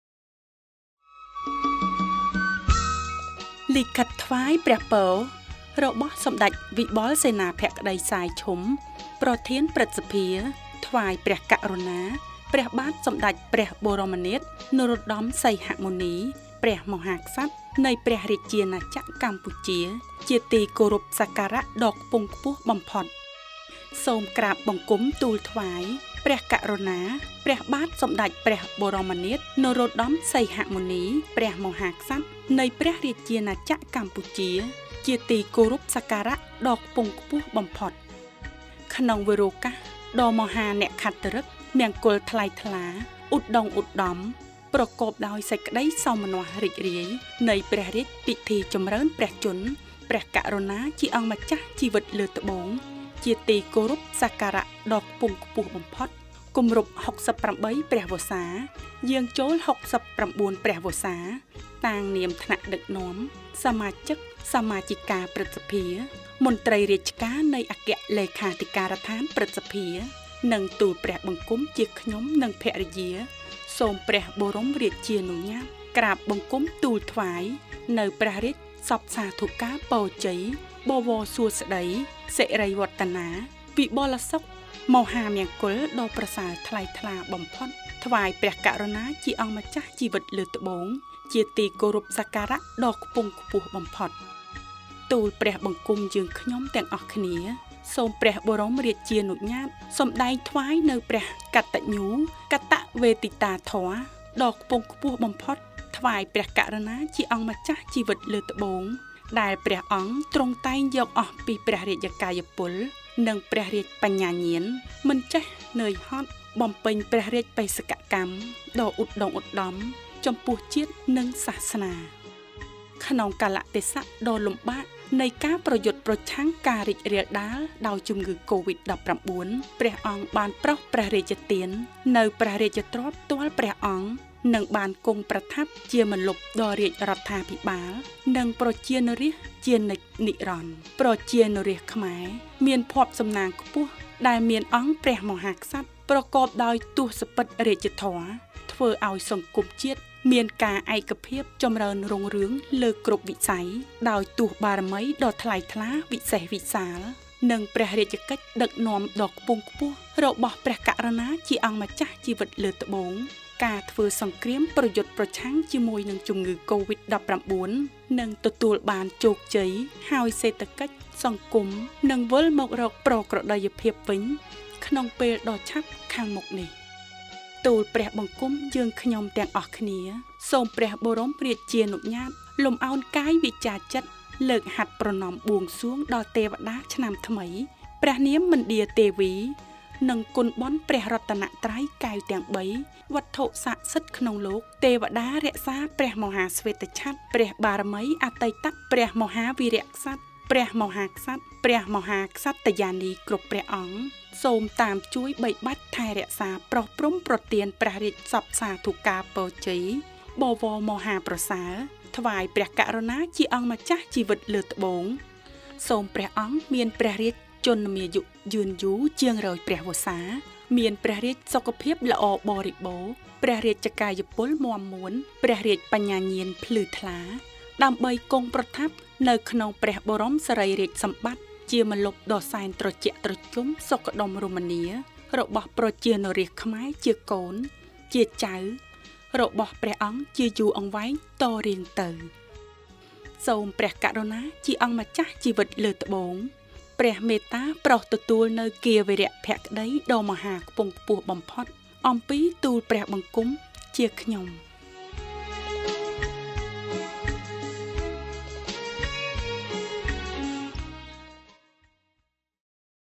11.05.2021 លិខិតថ្វាយព្រះពរព្រះមហាក្សត្រ នៃព្រះរាជាណាចក្រកម្ពុជា ក្នុងព្រះរាជពិធីបុណ្យចម្រើនព្រះជន្ម
លិខិតថ្វាយព្រះពររបស់សម្តេចវិបុលសេនាភក្តី សាយ ឈុំ ប្រធានព្រឹទ្ធសភា ថ្វាយព្រះករុណាព្រះបាទសម្តេចព្រះបរមនាថ នរោត្តម សីហមុនី ព្រះមហាក្សត្រនៃព្រះរាជាណាចក្រកម្ពុជា ក្នុងឱកាសព្រះរាជពិធីបុណ្យចម្រើនព្រះជន្មគម្រប់ ៦៨ព្រះវស្សា យាងចូល ៦៩ព្រះវស្សា។